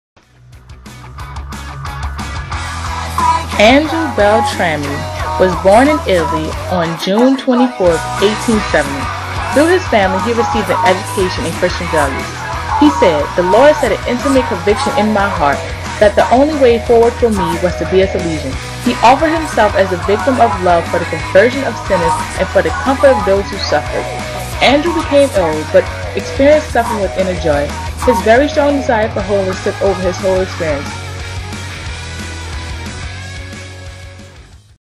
Andrew Beltrami
Short Clip on the life of this member of our Salesian Family by Academy of Our Lady Seniors